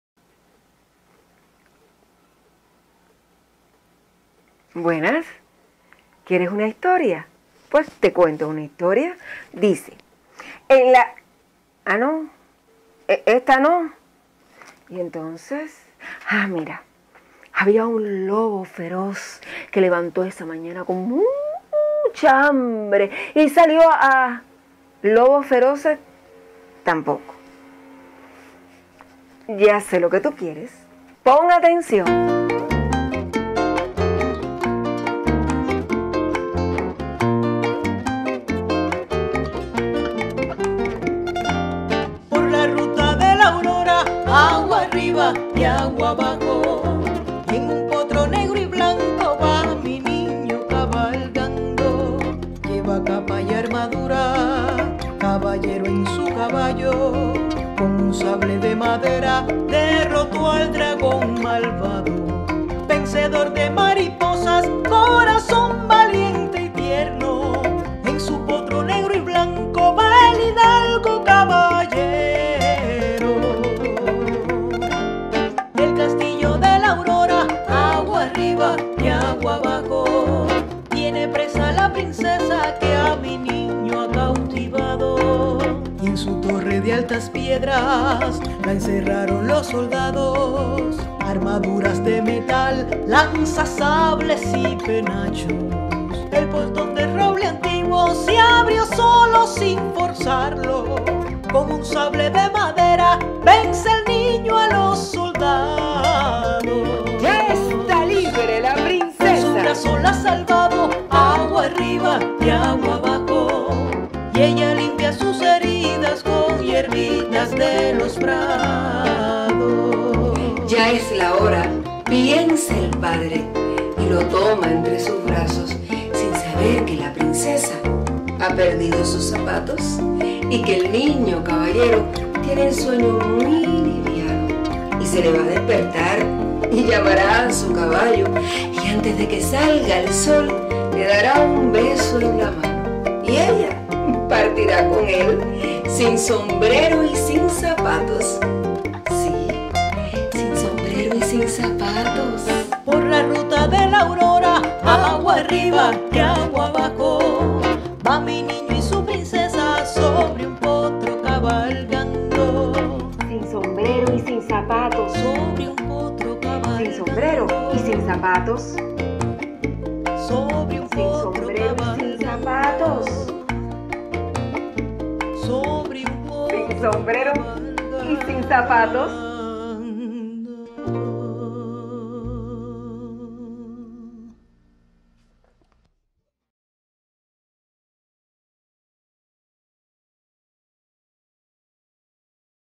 Mitin Poético Virtual: Porque mi niño me pidió un cuento de una princesa y un caballero
Actúan:
Nubelia Leyva Ferrer
Mirtha Rivero Bustamante